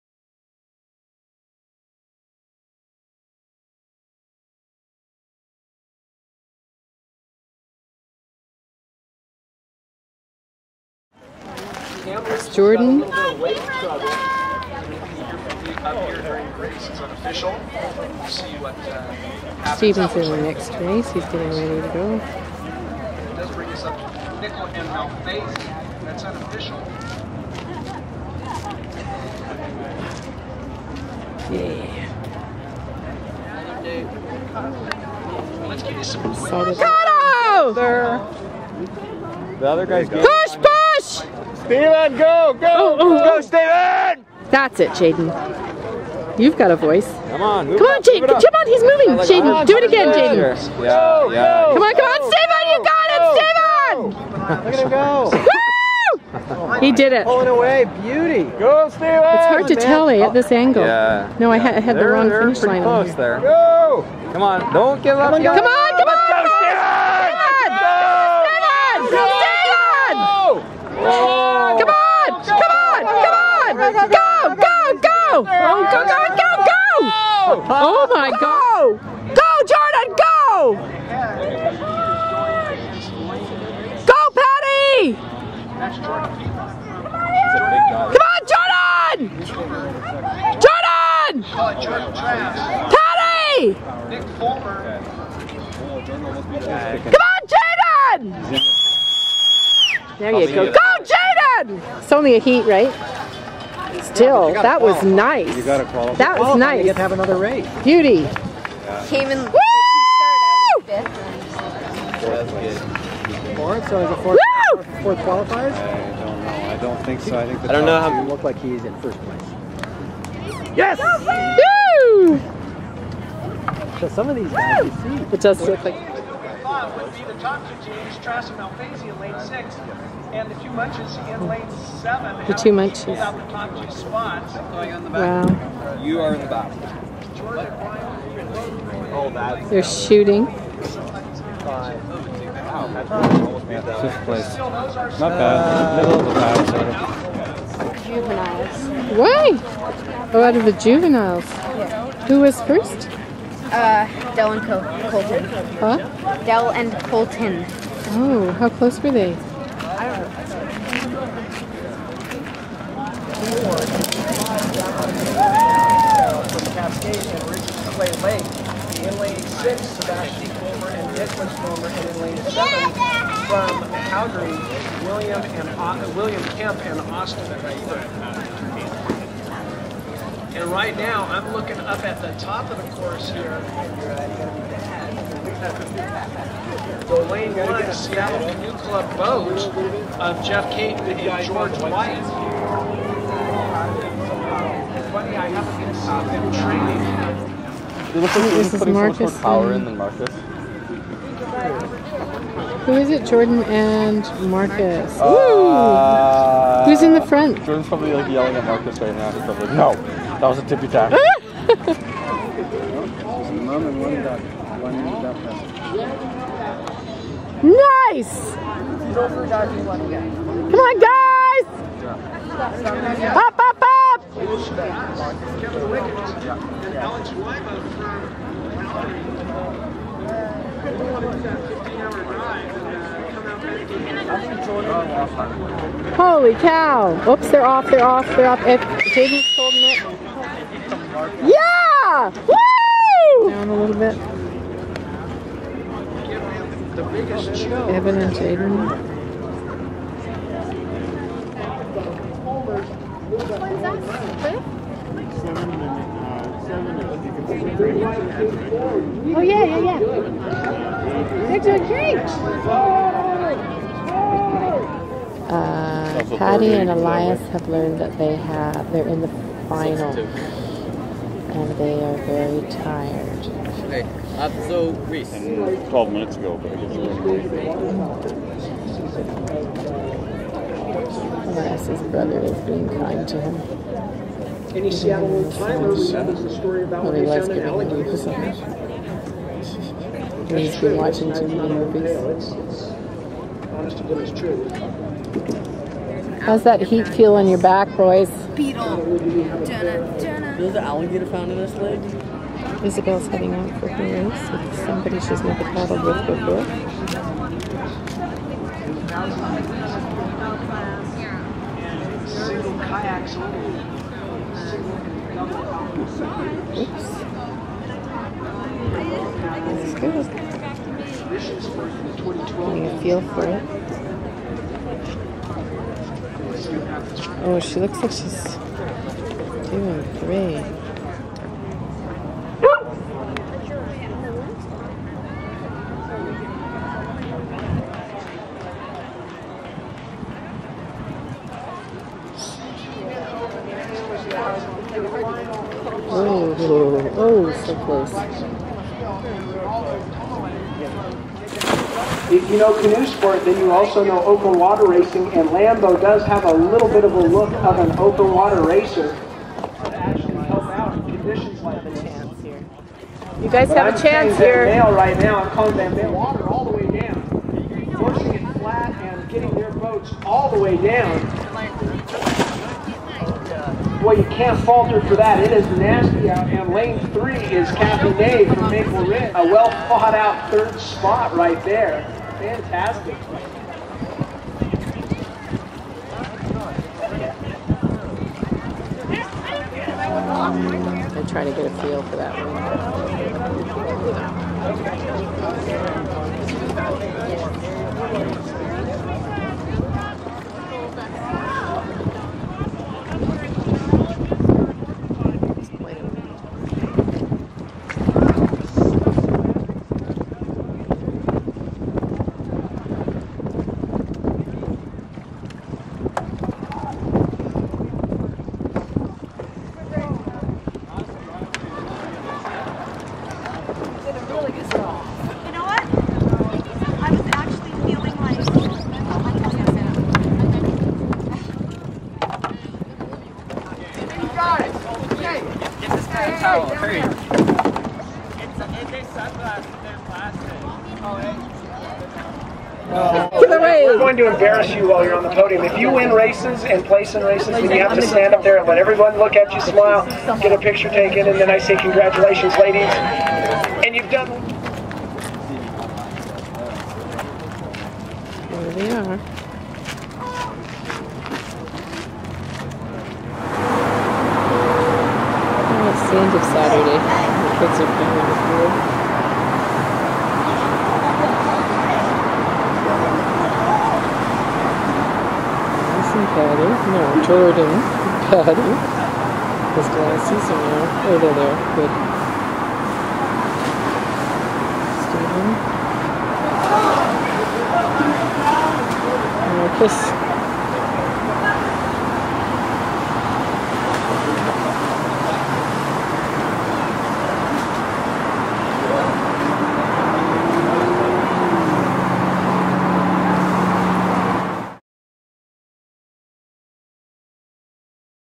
large.m4v
Some heats on Saturday, June 5th, at the 2010 Ted Houk Seattle Regatta for Victoria Youth Paddling Club Racers and Jr. Racers.